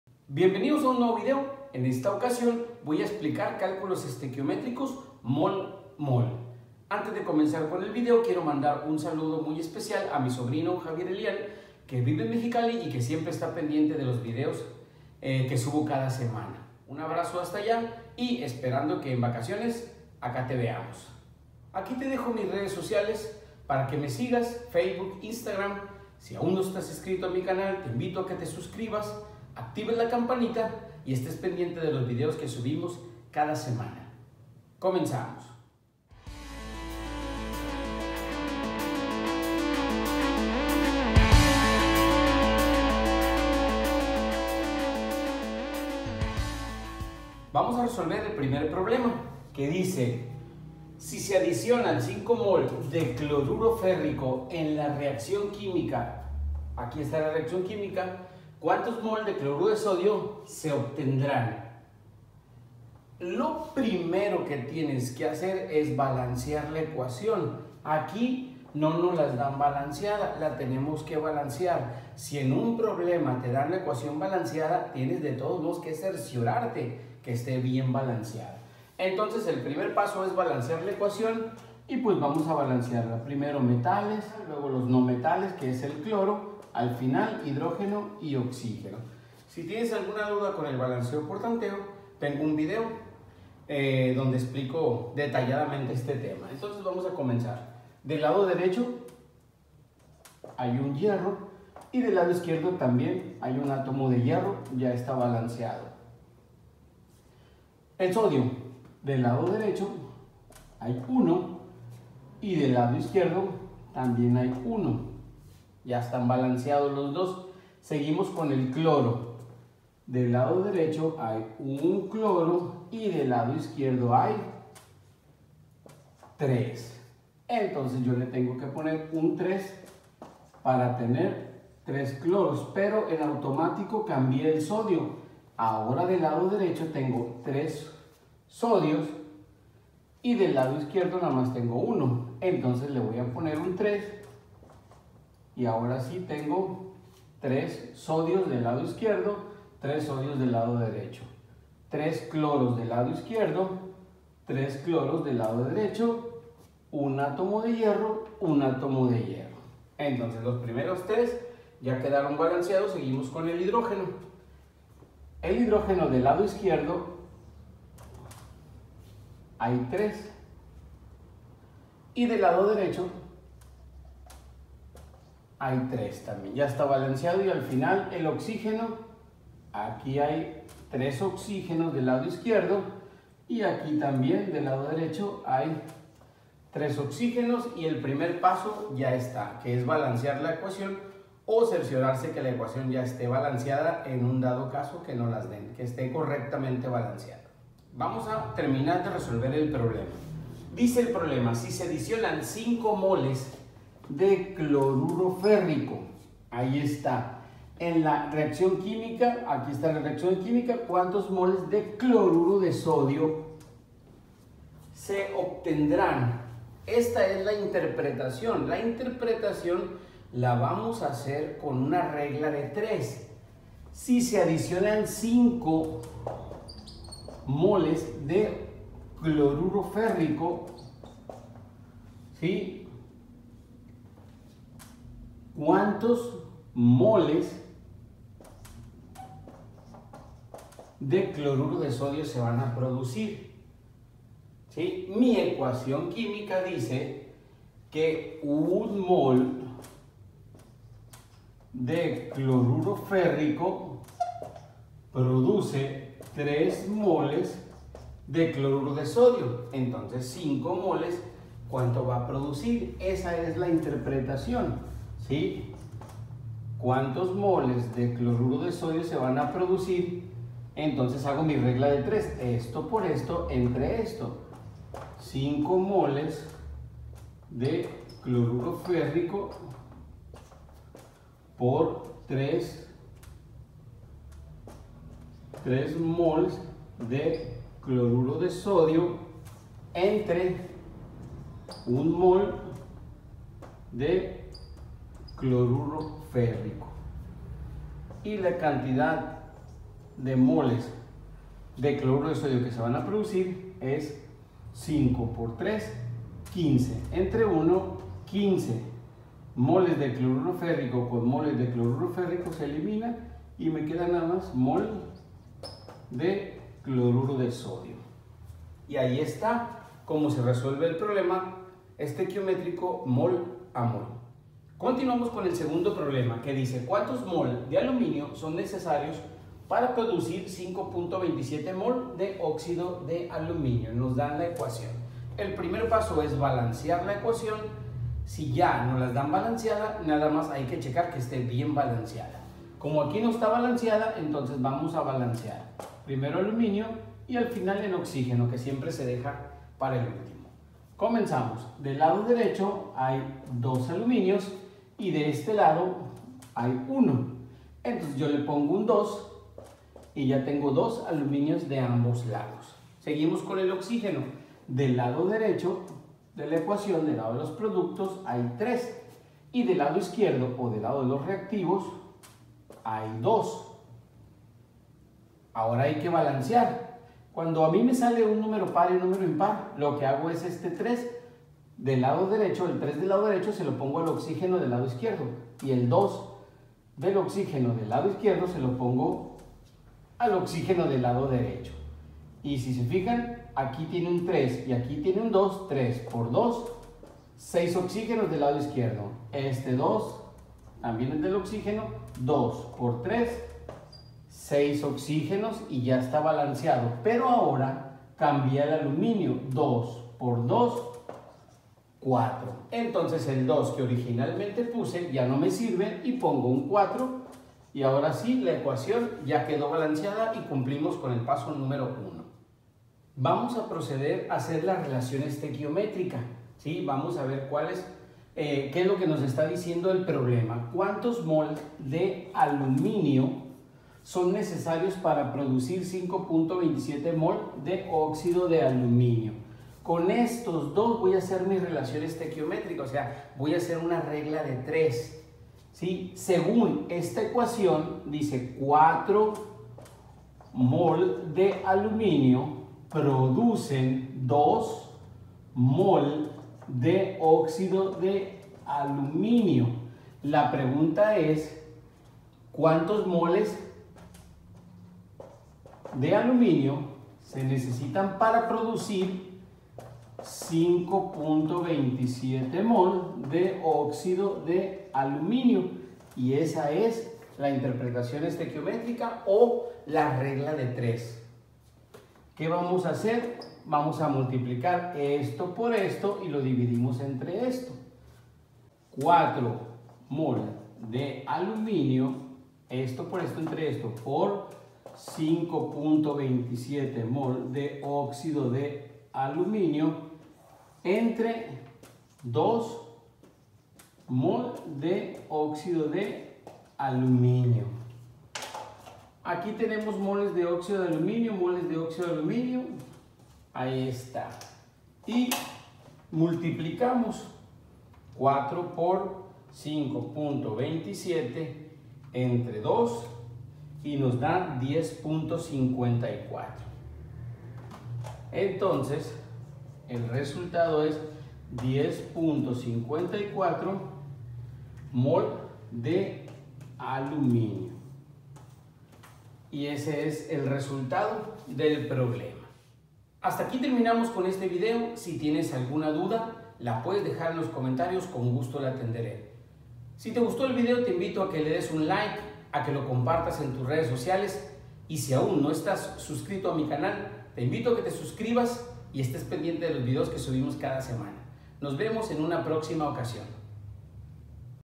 Cálculos Estequiométricos: Mol - Mol
Como realizar cálculos estequimétricos mol - mol